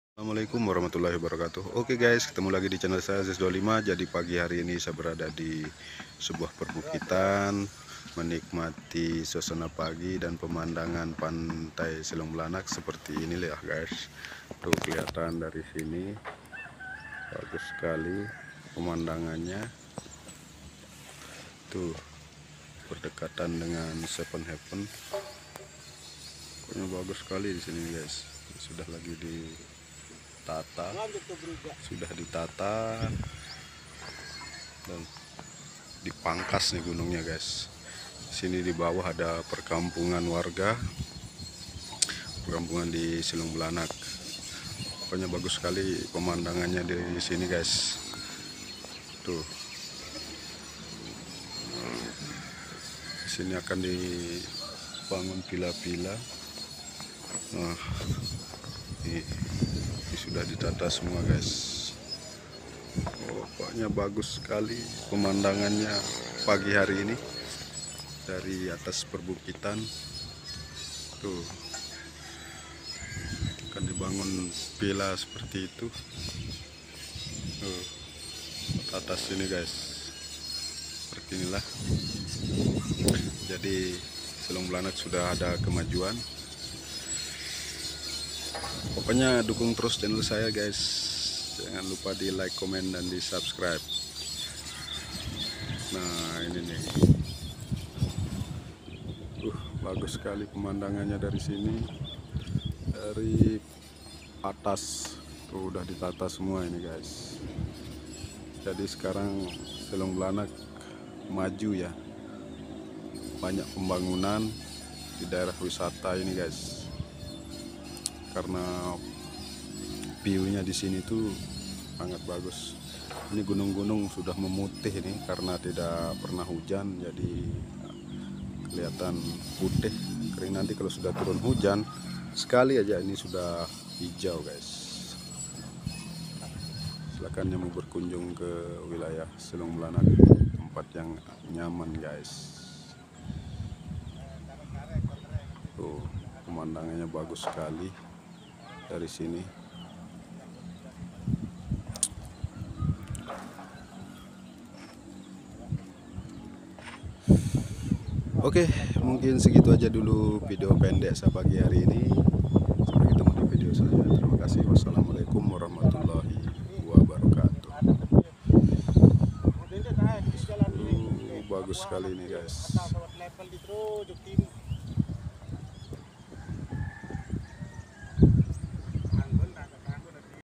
SEPERTI INILAH SUASANA DAERAH WISATA DISULAP MENJADI BAGUS
assalamualaikum apa kabar teman teman semoga tetap dalam keadaan sehat ya.

Nah gaes sekarang daerah wisata Selong belanak sudah disulap menjadi bagus semua bukit sudah ditata sama pemiliknya tinggal dibangun villa.